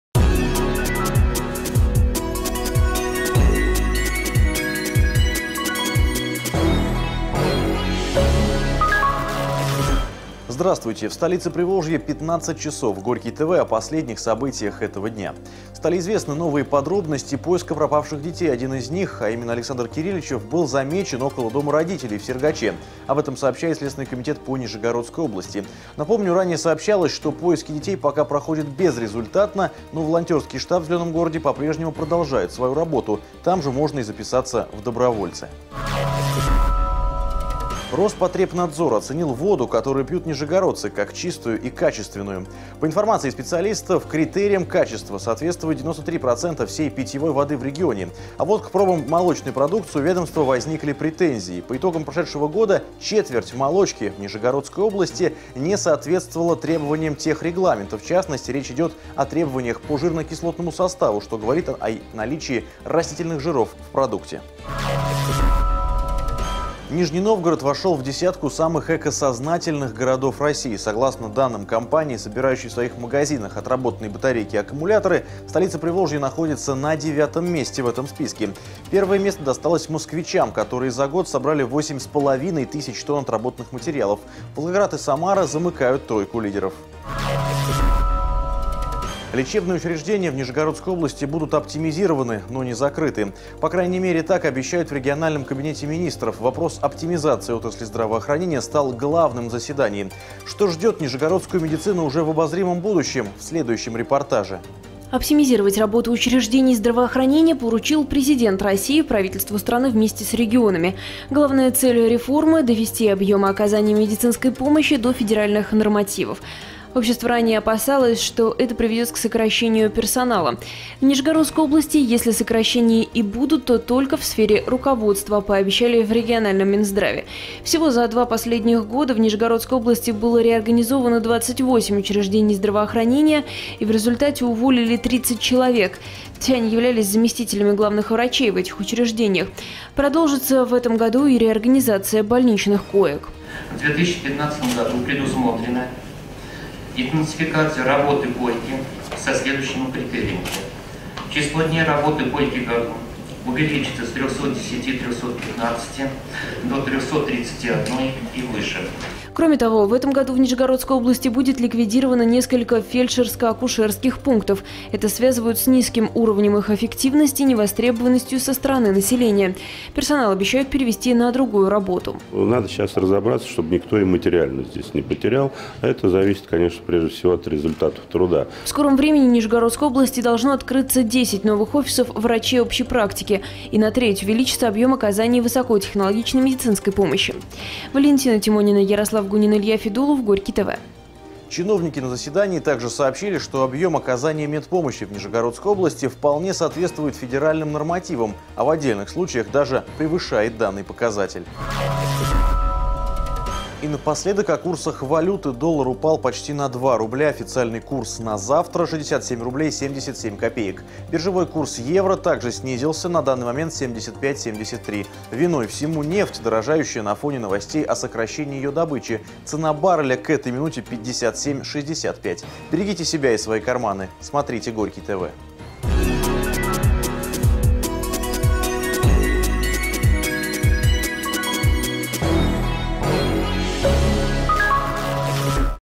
ОПТИМИЗИРУЮТ, НО НЕ ЗАКРЫВАЮТ
Один из пропавших мальчиков в Нижнем Новгороде замечен около дома родителей в Сергаче

Лечебные учреждения в Нижегородской области будут оптимизированы, но не закрыты

Роспотребнадзор оценил воду, которую пьют нижегородцы, как чистую и качественную

Нижний Новгород на 9 месте в рейтинге самых экосознательных городов России